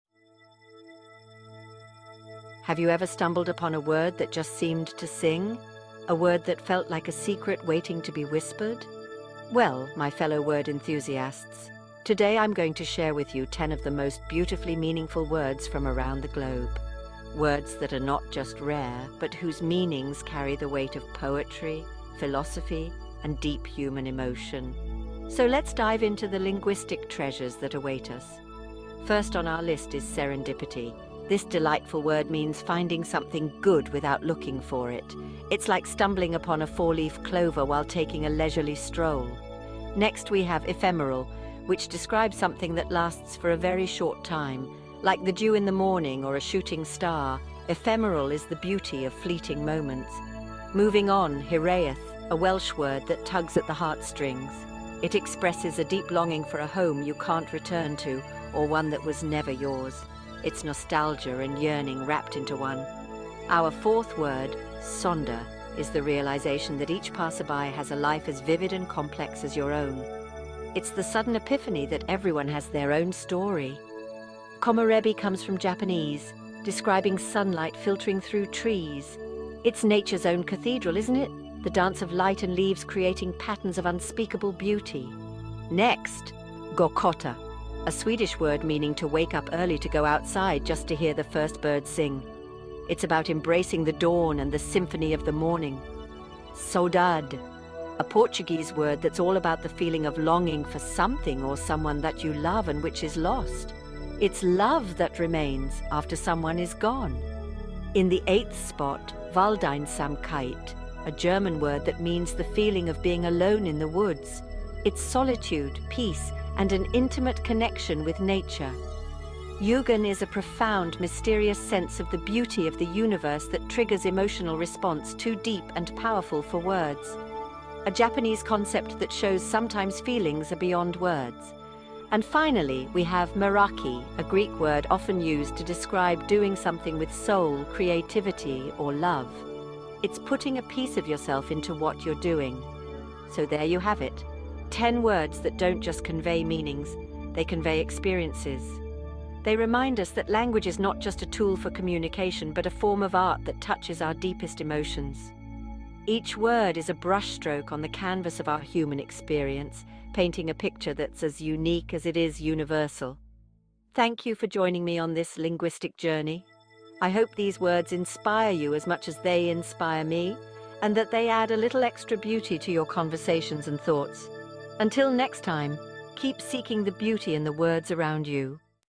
Rare Words With Beautiful Meanings
Here are 10 rare words with meanings so beautiful you will want to remember them every day.
Which words do you find exhilarating?

Thanks for every gesture ...